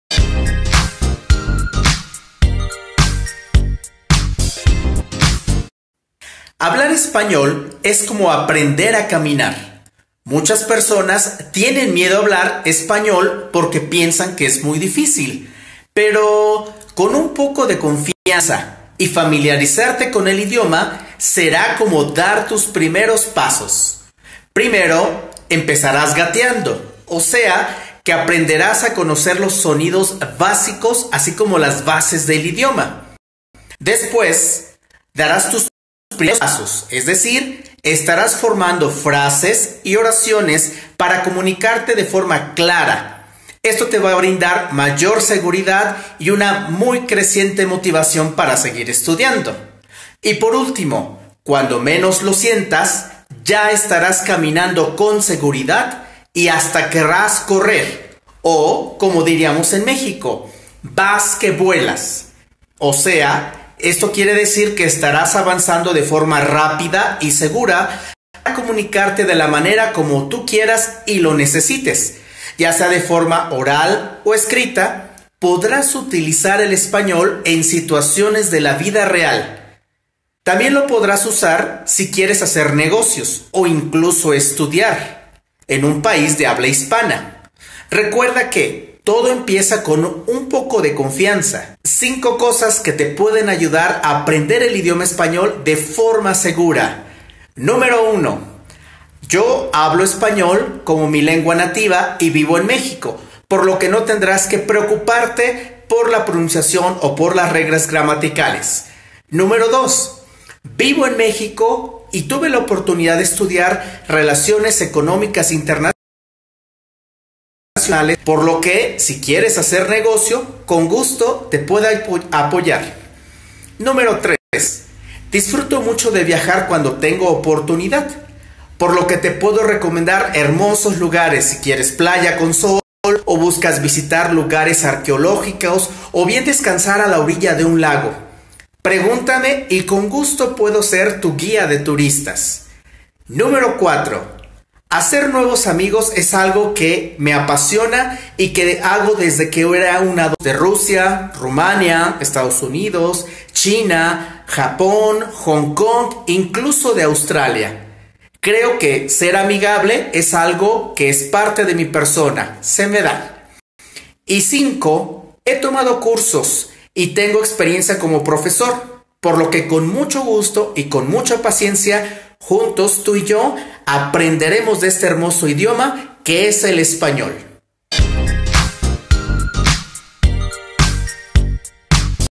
Español Nativo, César Miranda
To learn Spanish needs a bit of trust and to know the basic rules of the language.
Aprender español requiere de un poco de confianza y conocer las reglas básicas del idioma.

Primero empezaras a formar oraciones cortas, pero con la seguridad de que te estarás comunicando de forma clara.
First of all you´ll begin to talk or write short sentences, but surely you will somunicating to yourself in a secure way.

Después podrás comunicarte para hacer amigos, negocios, viajar o incluso estudiar en un país de habla hispana.
After this you will be able to make friends, make business, travel also study in a hispanic country.

Acerca de mi:
About me:

* Mi español es mi lengua materna.
* I speak Spanish as first language

* Vivo en Ciudad de México
* I live in Mexico city

* Estudié Relaciones Económicas Internacionales en la universidad.
* I´ve a degree in International Economic Relations at High School

* Me gusta hacer amigos de diferentes partes del mundo
* I like to make friends from diferents countries of the world

* Me considero amigable y paciente para enseñar.
* I think of my self I am friendly and patient to teach

Agenda conmigo tu clase muestra
Ask for your free class

César